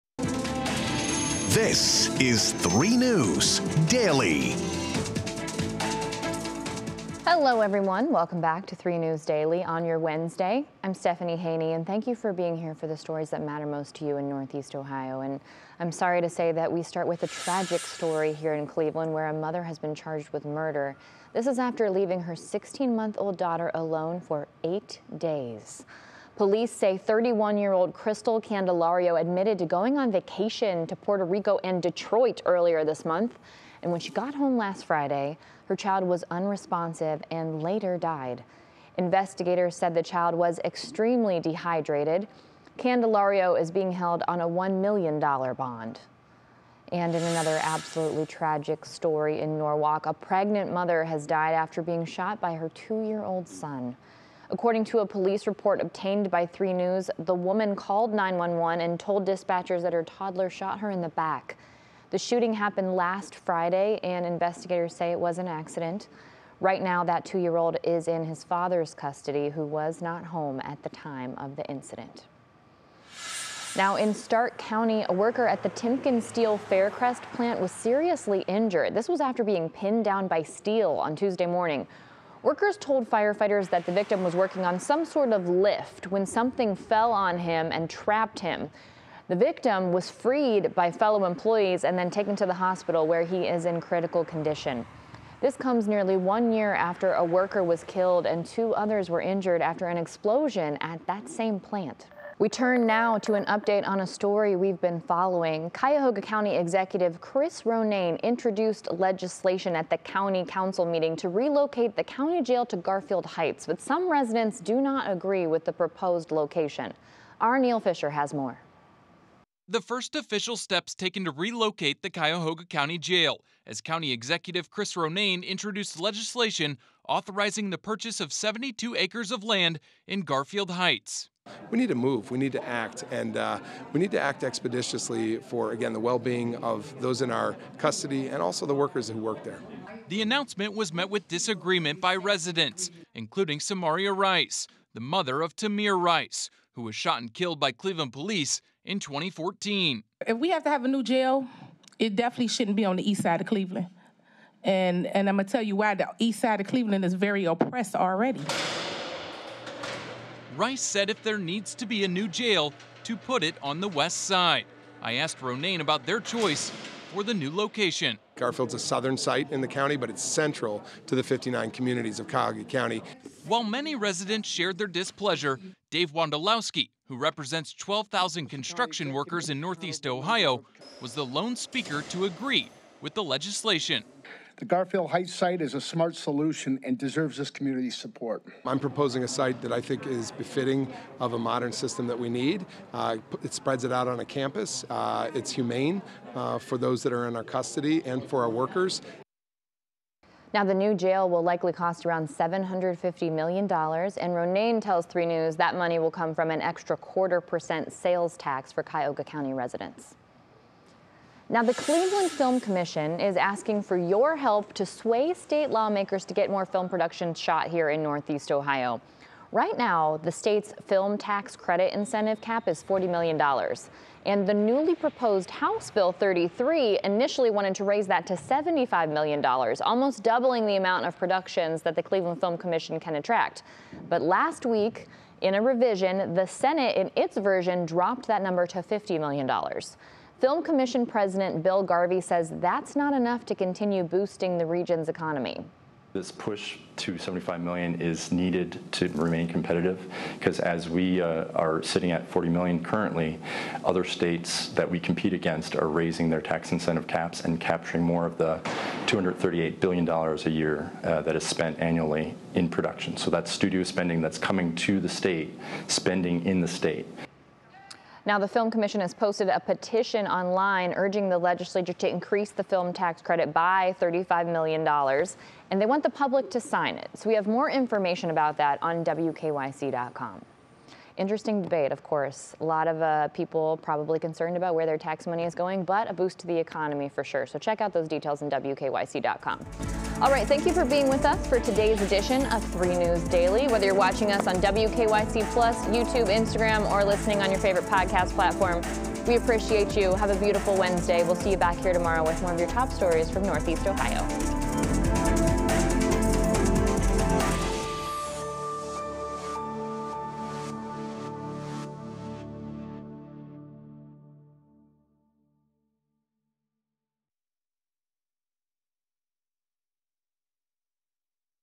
Baby dies while Cleveland mother charged with murder was gone on vacation to Puerto Rico and Detroit
Wednesday, June 21, 2023: A 16-month-old baby has died while her mother, now charged with murder, left her alone in Cleveland while she was gone on an 8-day vacation to Puerto Rico and Detroit. In another horrific story, a pregnant mother was accidentally shot and killed by her 2-year-old son. In Stark County, a worker was seriously injured at the TimkenSteel Faircrest plant, while Cuyahoga County is now considering legislation to put a new jail in Garfield Heights. Plus, the Cleveland Film Commission is asking for voters’ help to convince Ohio lawmakers to increase the filming tax credit cap by $35 million to boost the economy, and more on 3News Daily with Stephanie Haney.

Cleveland police: 16-month-old girl dies after mother left her alone for more than a week to go on vacation